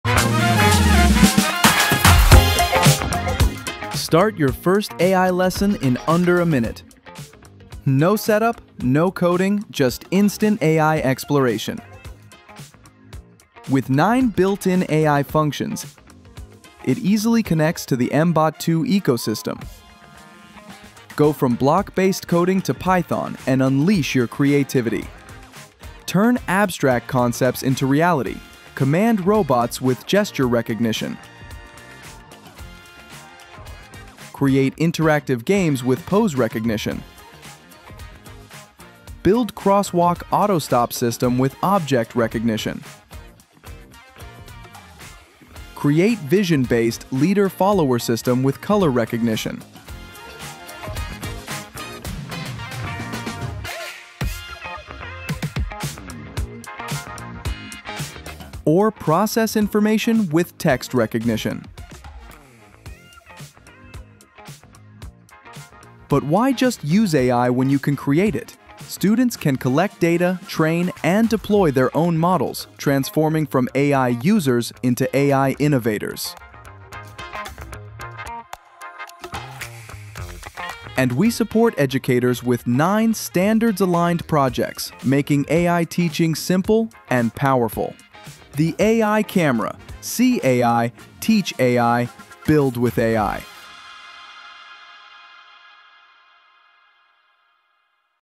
AI Camera 2.0: The All-in-One AI Vision Module for Education
Meet the AI Camera 2.0, the all-in-one vision module made for education. Go from unboxing to discovery in seconds with 9 built-in AI functions. Connect seamlessly to mBot2, program your creations, and empower students to train their own models. From first sight to future skills—see AI, teach AI, and build with AI.